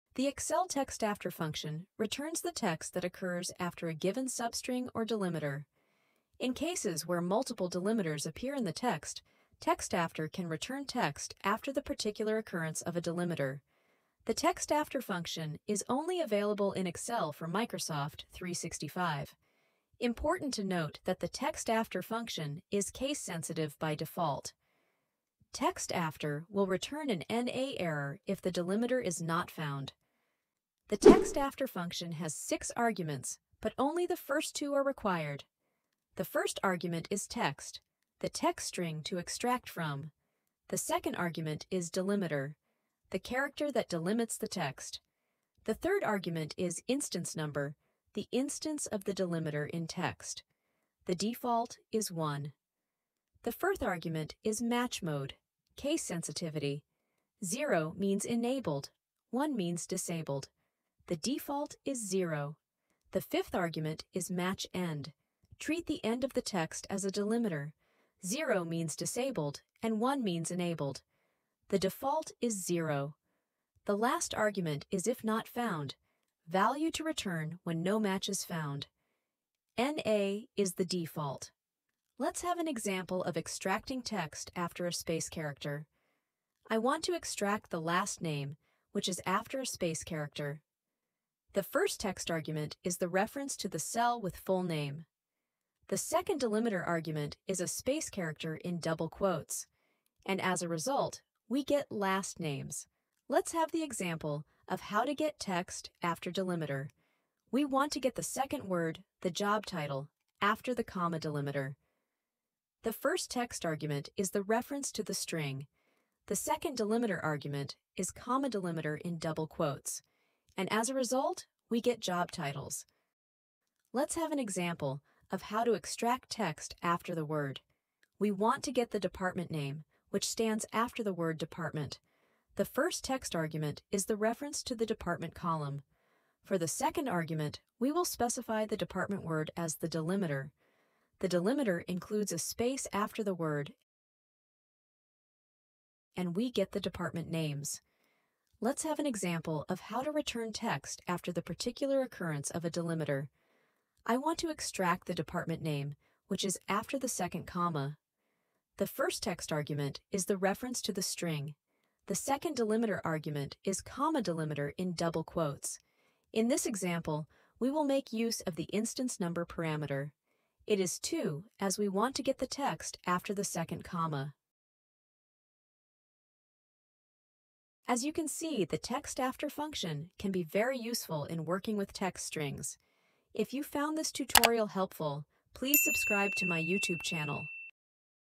How To Use the Excel TEXTAFTER Function?
The Excel TEXTAFTER function returns the text that occurs after a given substring or delimiter. In cases where multiple delimiters appear in the text, TEXTAFTER can return text after the particular occurrence of a delimiter.
The TEXTAFTER function is only available in Excel for Microsoft 365.